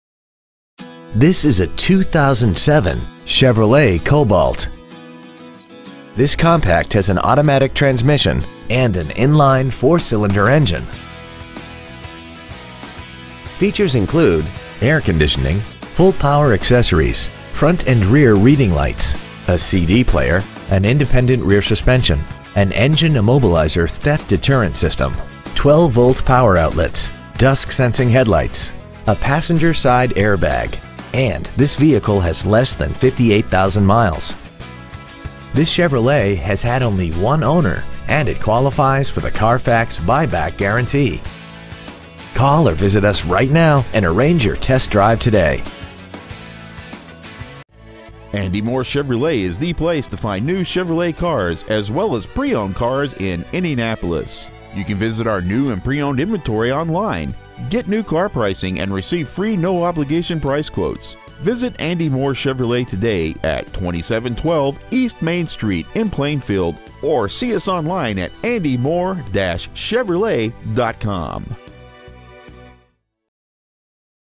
2007 Chevrolet Cobalt Indianapolis IN 46168
Year : 2007
 Make : Chevrolet
 Model : Cobalt
 Engine : 2.2L I4 16V MPFI DOHC
 Trans . : Automatic
 Exterior : Ultra Silver Metallic
 Miles : 57,330
 Interior : Gray
 Stock : PT1513C

 2712 E. Main Street